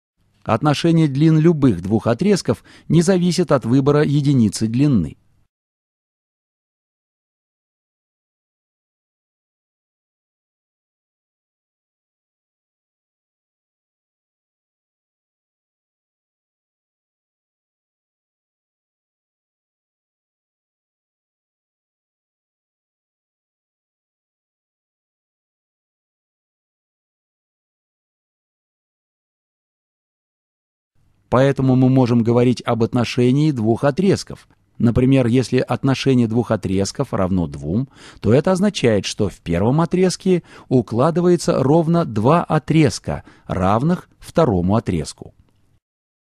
Отношение длин отрезков
Отношение длин любых двух отрезков не зависит от выбора единицы длины. Поэтому мы можем говорить об отношении двух отрезков. Например, если отношение двух отрезков равно двум, то это означает, что в первом отрезке укладывается ровно два отрезка, равных второму отрезку.